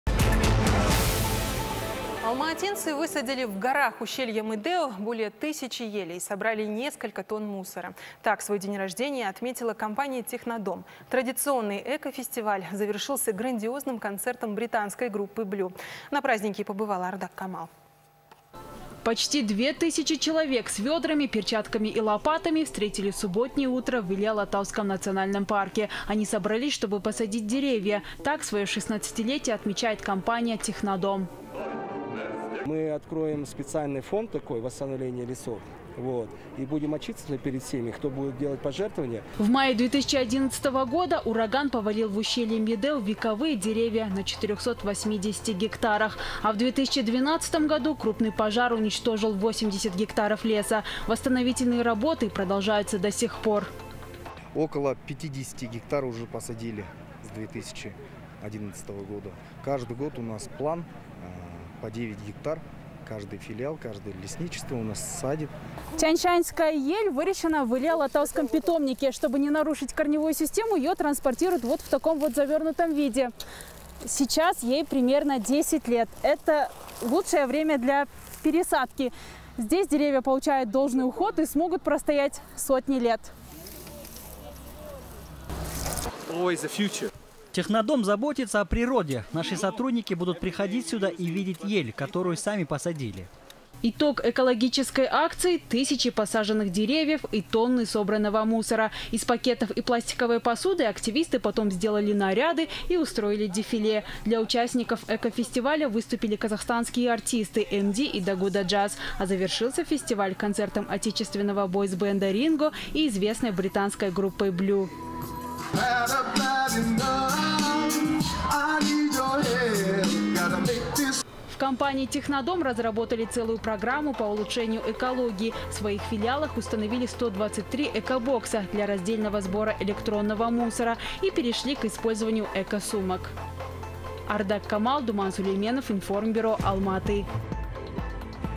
Компания «Технодом» отметила свой день рождения
Алматинцы высадили в горах ущелья Медеу более тысячи елей и собрали несколько тонн мусора. Так свой день рождения отметила компания «Технодом». Традиционный эко-фестиваль завершился грандиозным концертом британской группы Вlue.